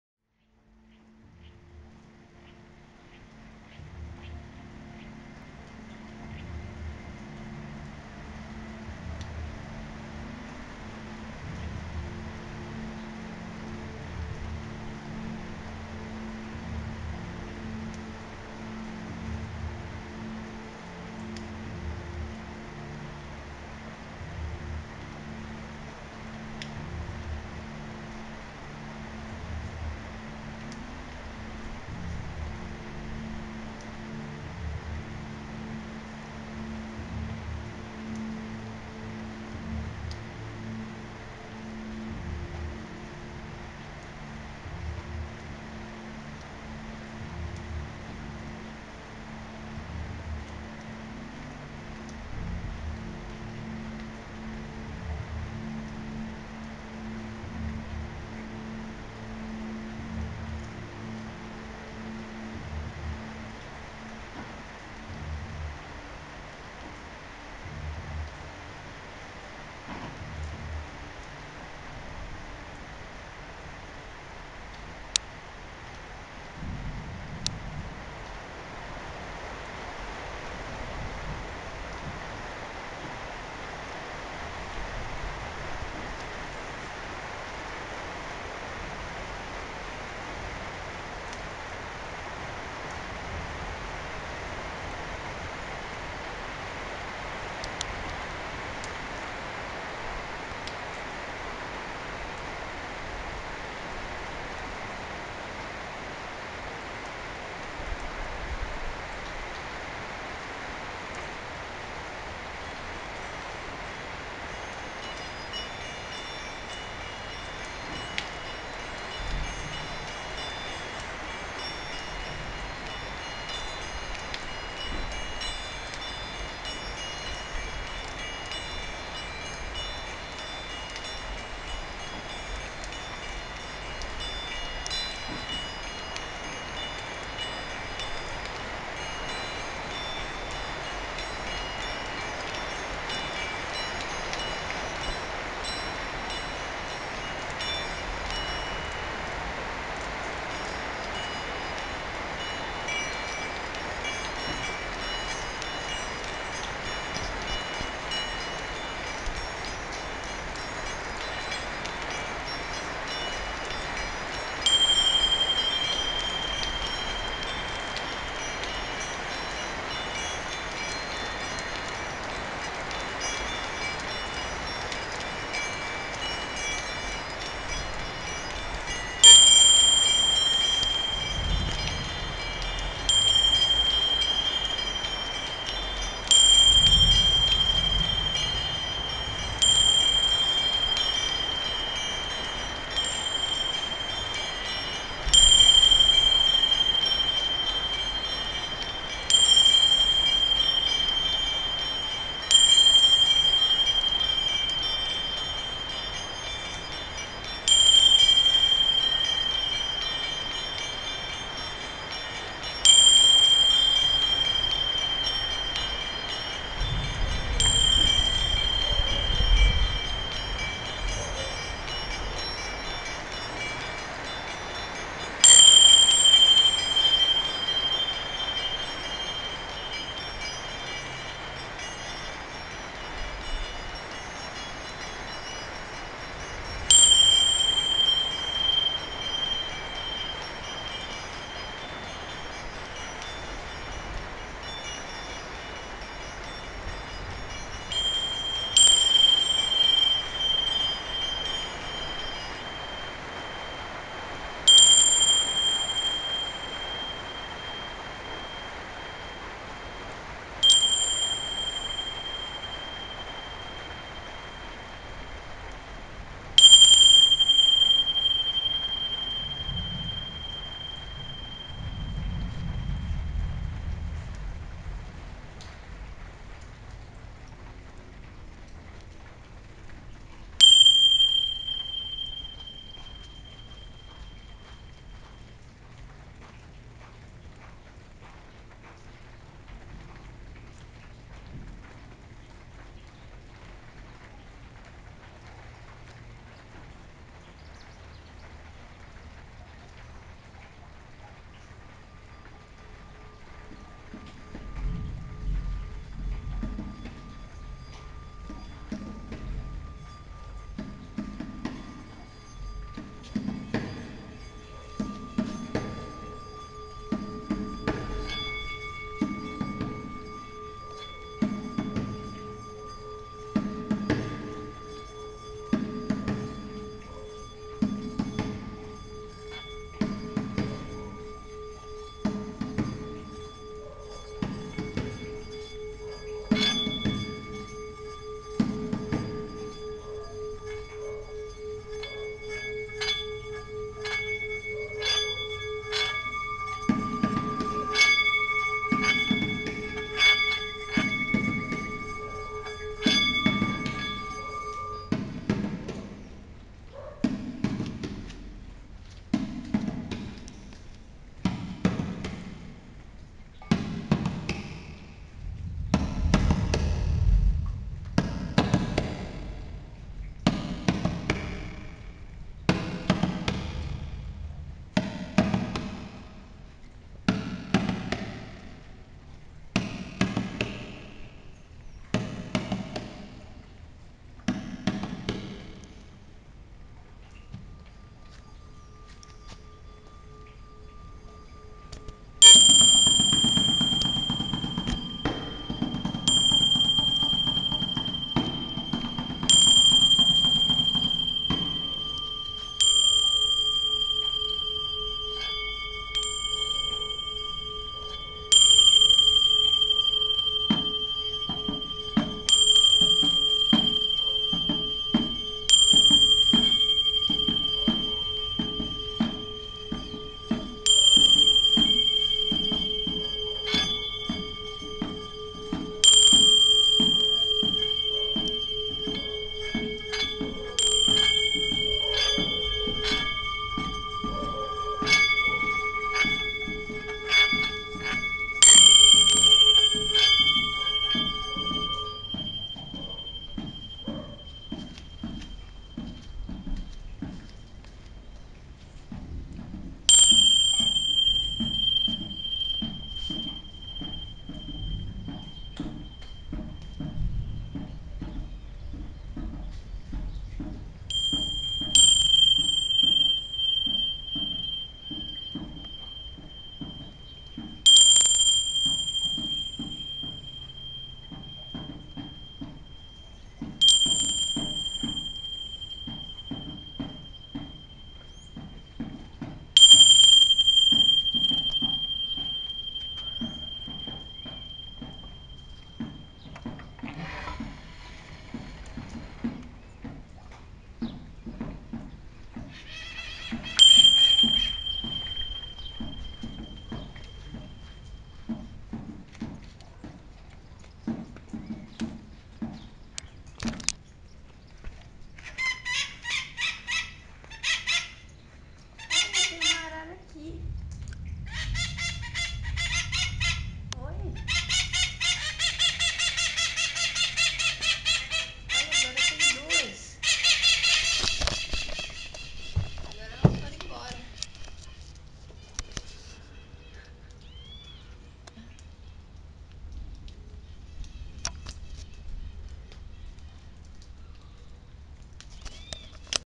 Abertura
Filme meditativo // registro de abertura de consciência
Pindamonhangaba, SP - 2017
Por Débora Guerreiro e Clara Chroma